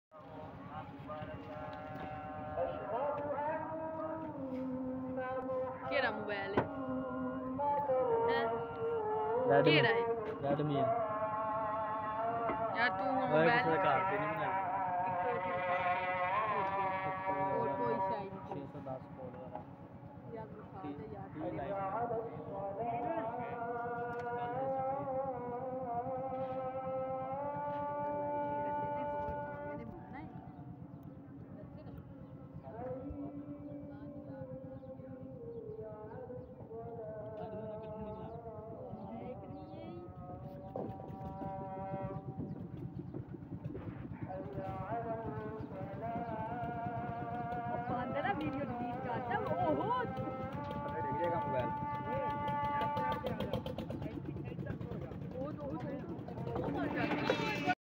How to make a roof in Punjab
Roof filling in Pakistan
Fixing of Bricks in Roof filling
Roof Constructing Work
This vedio has free copyrights